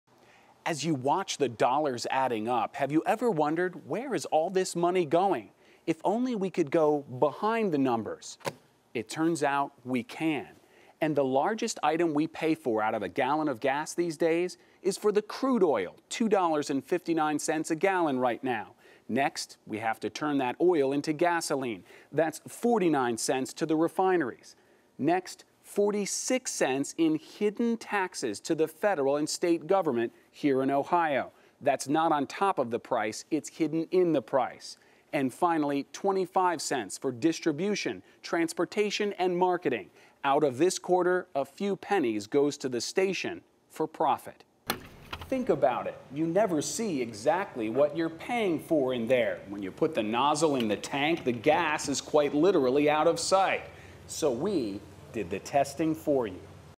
MMJ Stand-Ups on Gas Prices
Anchor/MMJ Brendan Keefe uses the iPad 2 to demonstrate the components of gas prices, plus the old Pringles can trick to put the camera "in the gas tank"